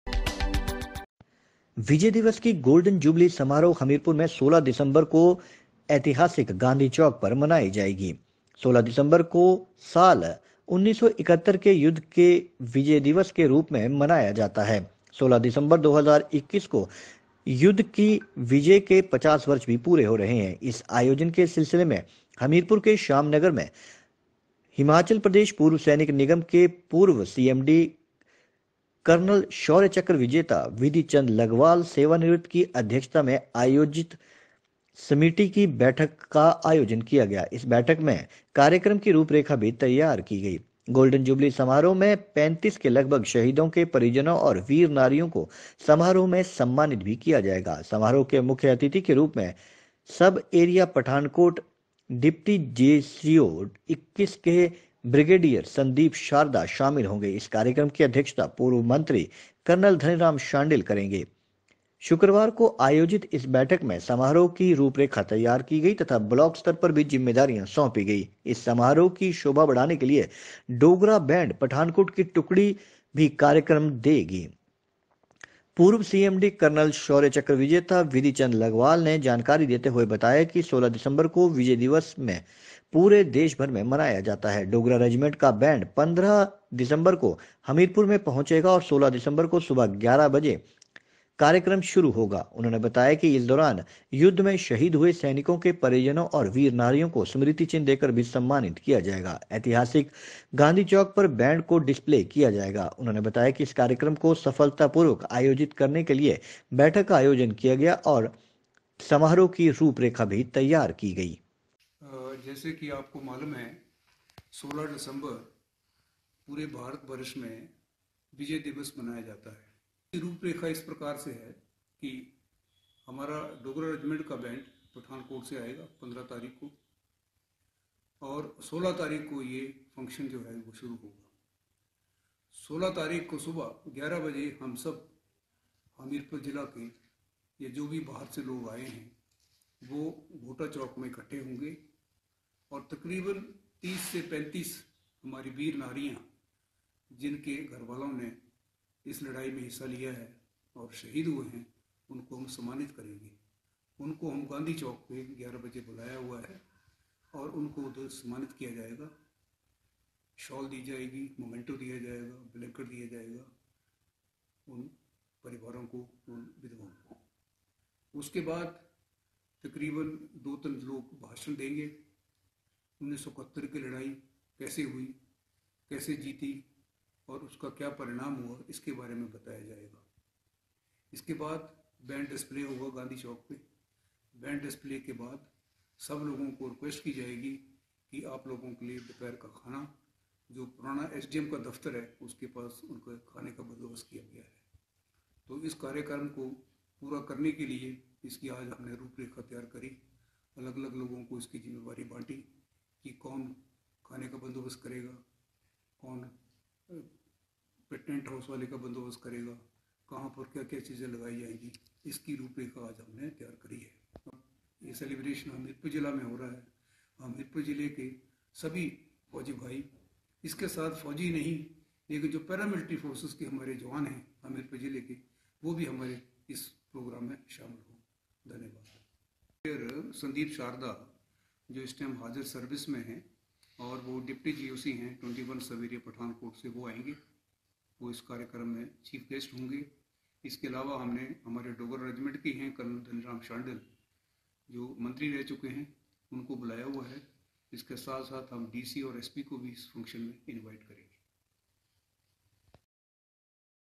Golden Jubilee celebrations of Vijay Diwas will be celebrated in Hamirpur
विजय दिवस की गोल्डन जुबली समारोह हमीरपुर में 16 दिसंबर को हमीरपुर के ऐतिहासिक गांधी चौक पर मनाया जाएगा। 16 दिसंबर को साल 1971 के युद्ध के विजय दिवस के रूप में मनाया जाता है। 16 दिसंबर 2021 को युद्ध की विजय के 50 वर्ष पूरे होंगे। इस आयोजन के सिलसिले में हमीरपुर के श्याम नगर में शुक्रवार को हिमाचल प्रदेश पूर्व सैनिक निगम के पूर्व सीएमडी कर्नल शौर्य चक्र विजेता विधीचंद लगवाल ( सेवानिवृत्त) की अध्यक्षता में आयोजन समिति की एक बैठक आयोजित हुई।